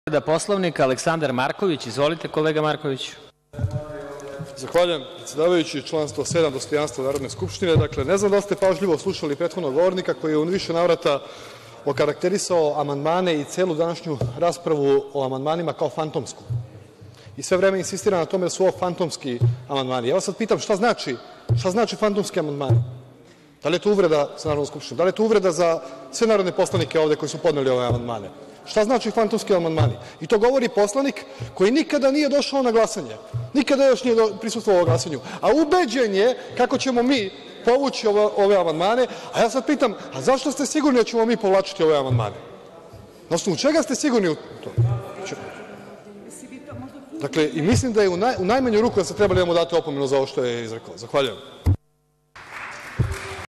Aleksandar Markovic, SNS, Po poslovniku, 12.03.2018. godine
Narodni poslanik SNS Aleksandar Markovic ukazuje na povredu Poslovnika o radu Narodne Skupstine od strane narodnog poslanika Djordja Vukadinovica, na sednici Narodne Skupstine, 12. marta 2018. godine.